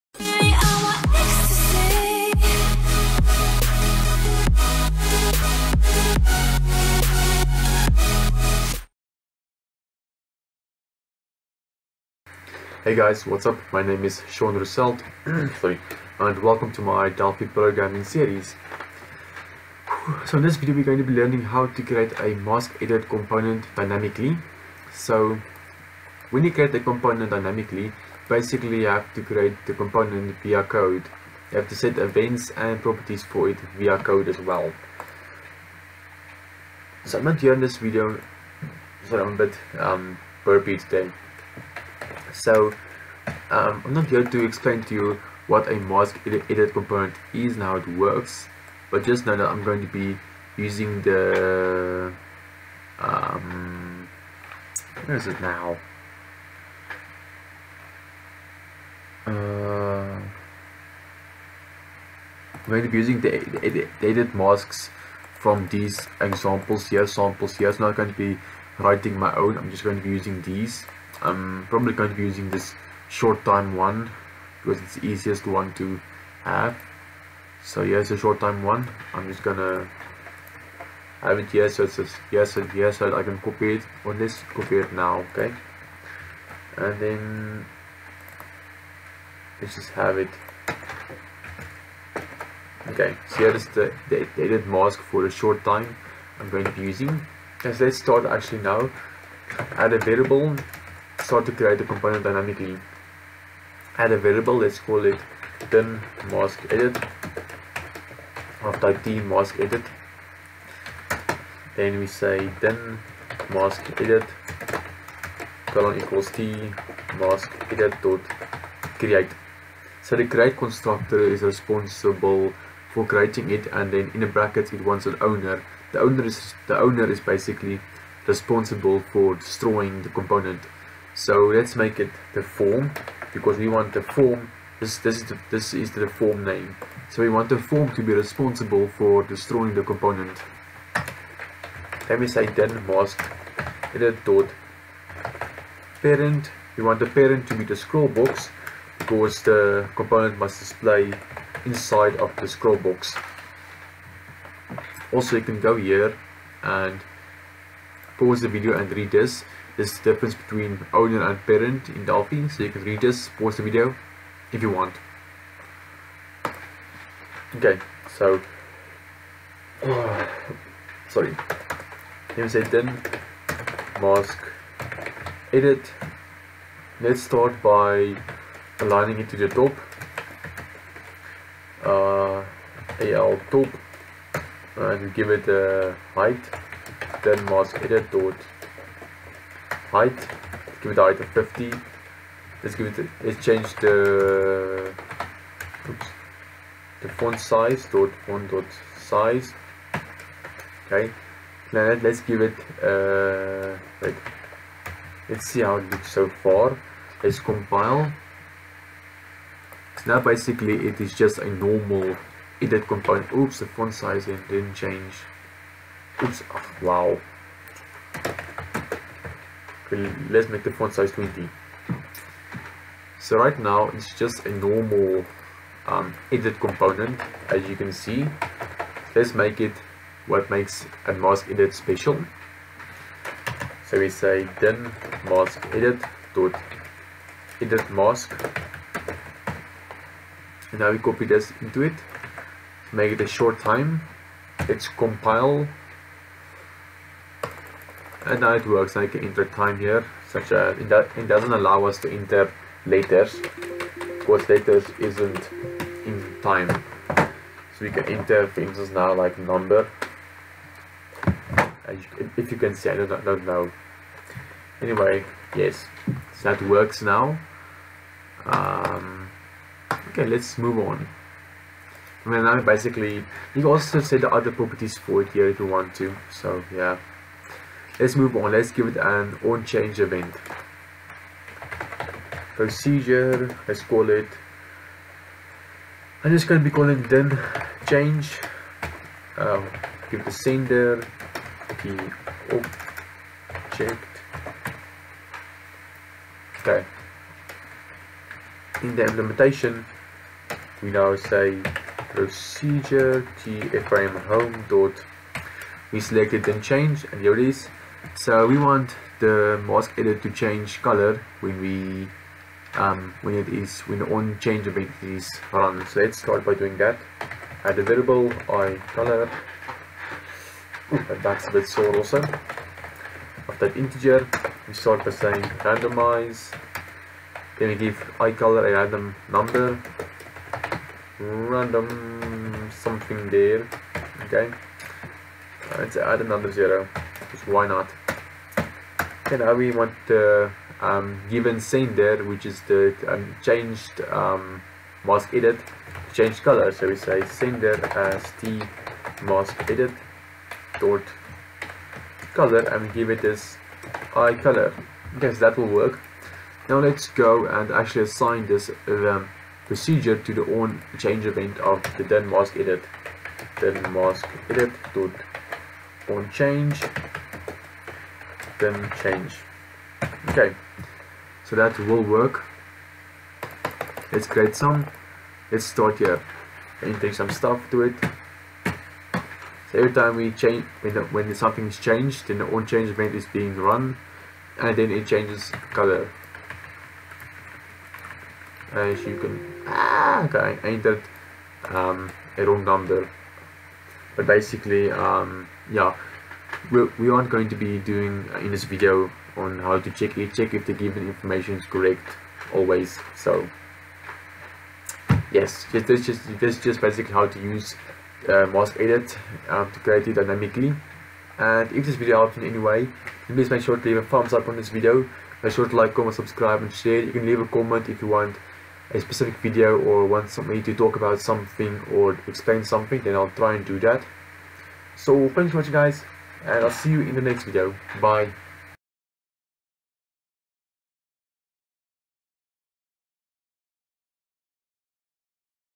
Delphi Programming Series: 48.12 - Dynamically creating a MaskEdit component
Hi. In this video I teach you how to create a MaskEdit component dynamically in run-time using the Delphi programming language.